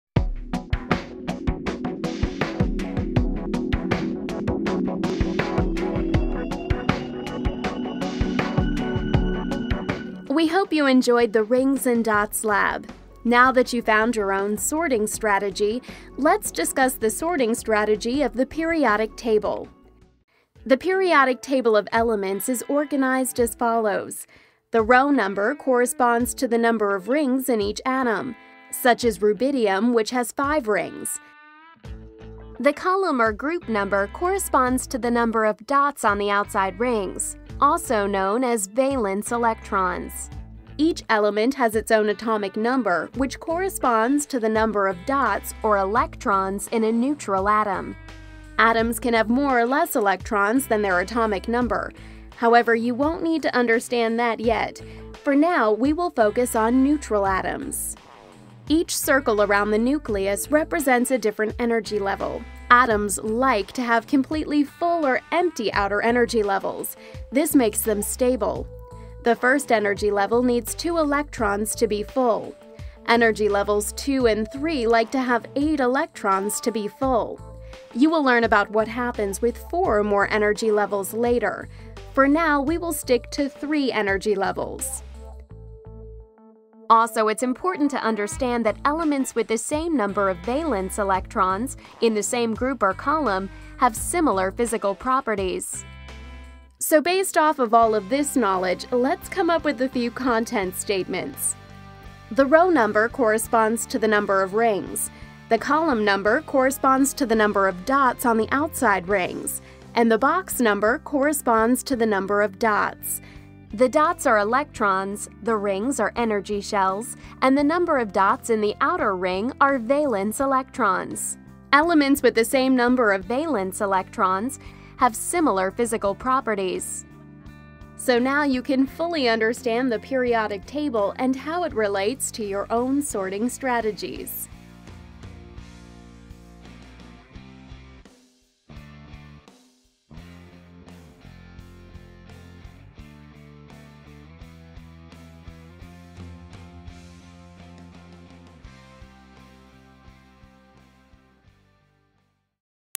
SC.8.P.8.6 Rings and Dots
Covers the atomic theory at an 8th Grade level. Explains how the periodic table is organized.

Florida Sunshine State Standard SC.8.P.8.6:
Recognize that elements are grouped in the periodic table according to similarities of their properties.